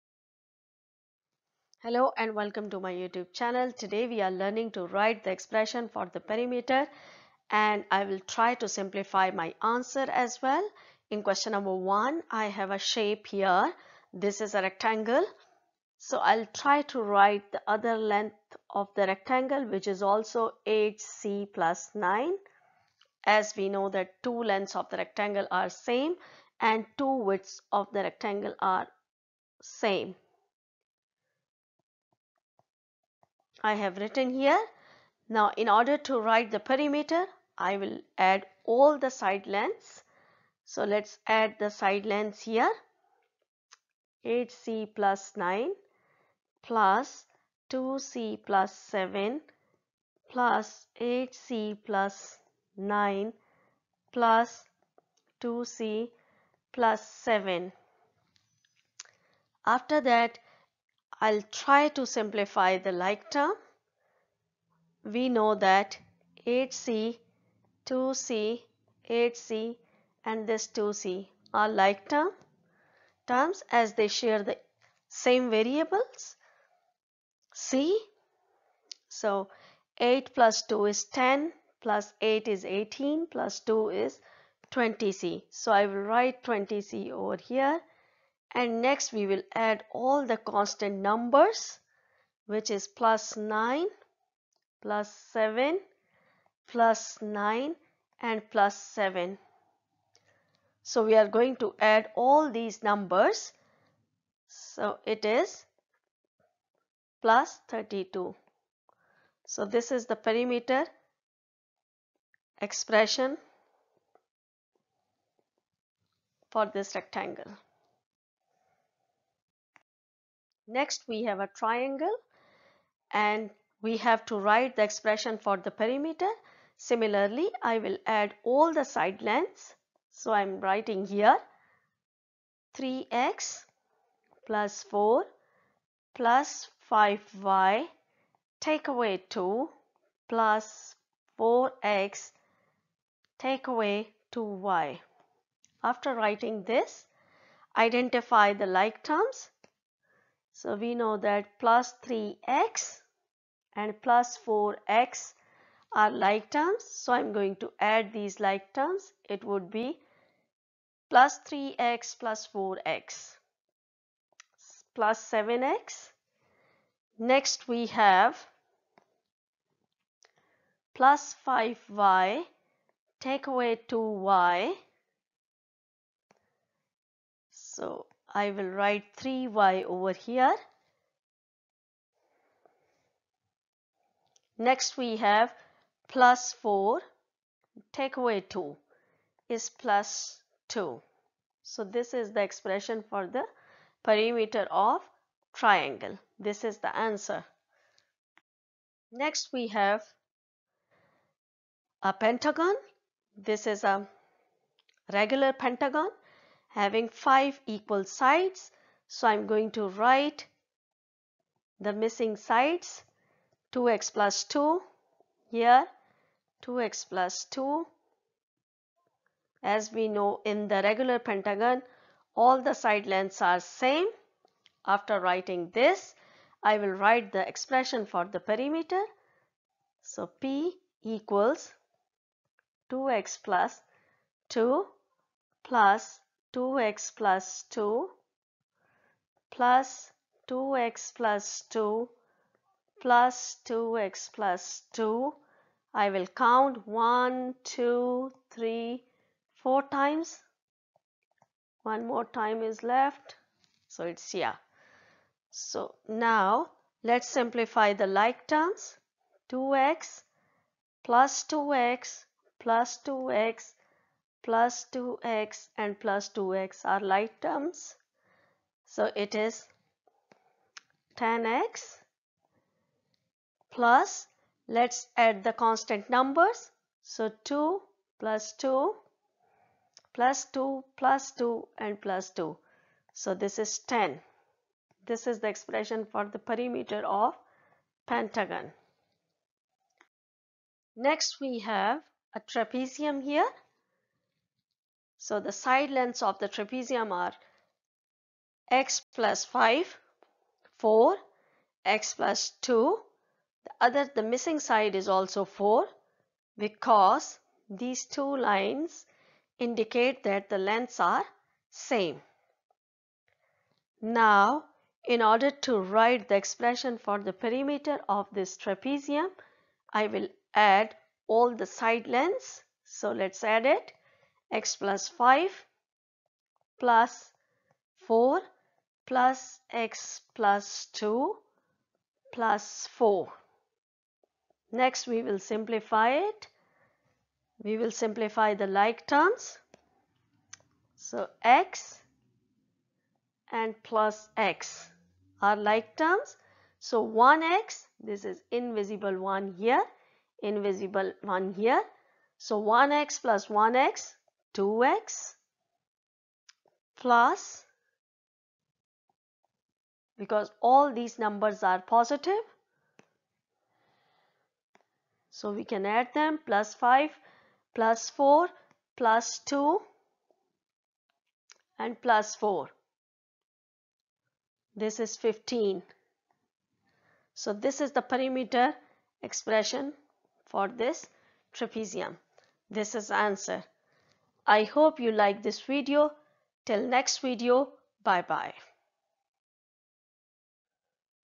Simplifying algebraic expression: Writing the perimeter expression
This video is about writing the perimeter of simple geometric shapes, triangles, rectangles, pentagon, trapezium. I hope you like this video.